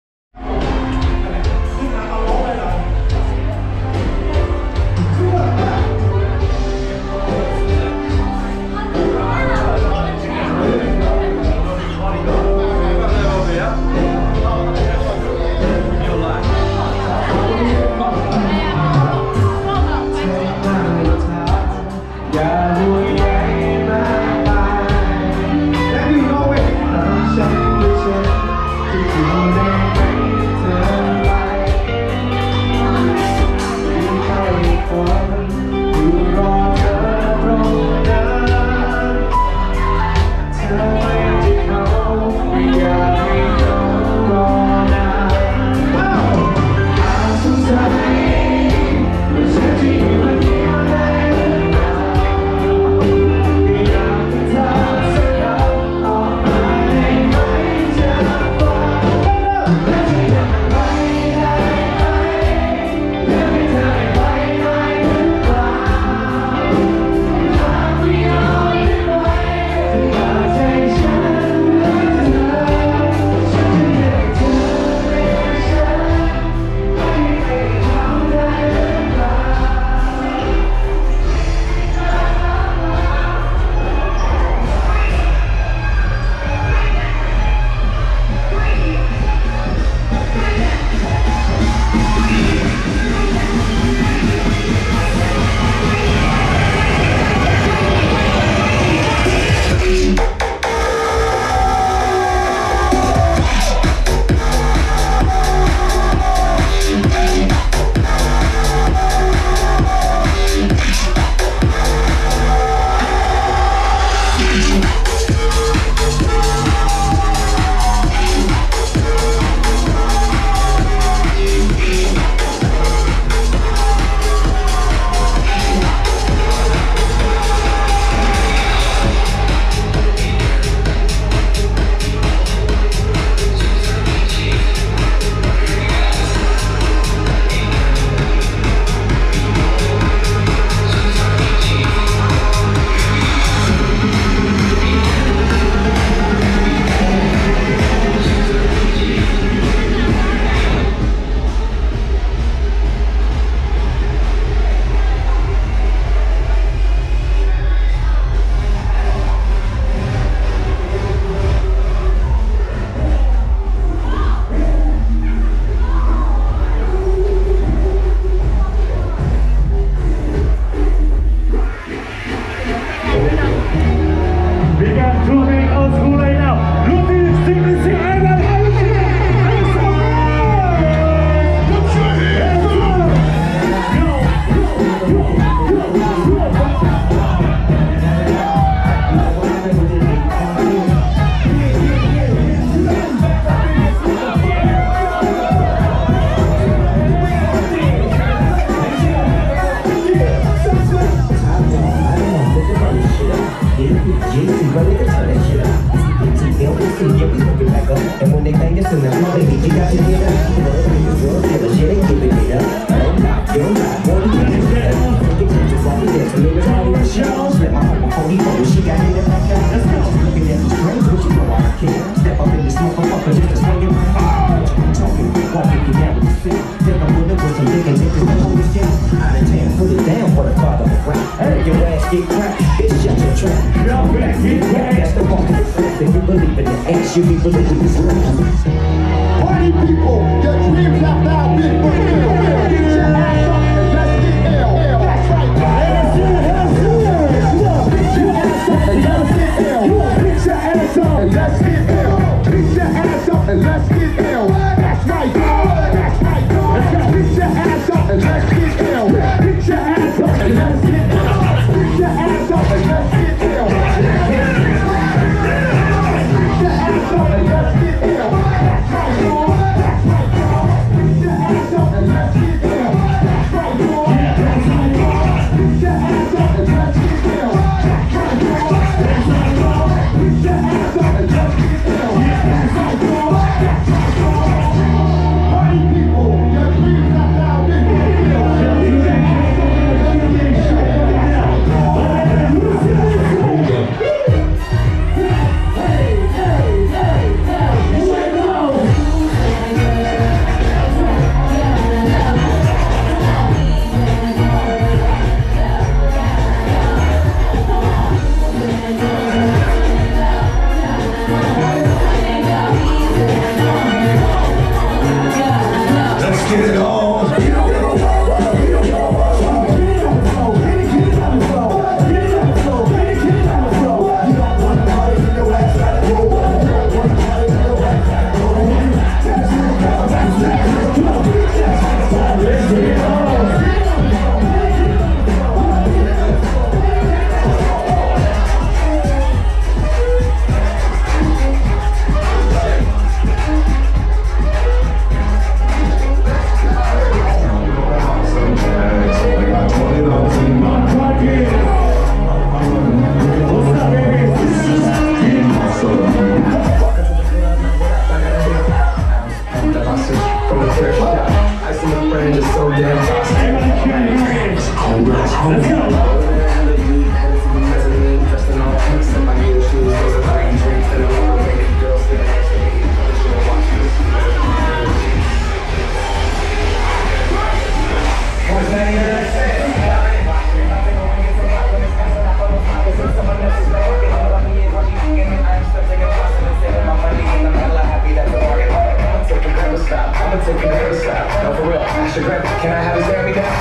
Route66 GoPro Max (ทดสอบถ่ายกลางคืนภาพเสียง 360 องศา แบบ VR
ทดสอบการตั้งค่าใหม่จากงานเดิมที่เกินปัญหาภาพกระพริบ ภาพสั่น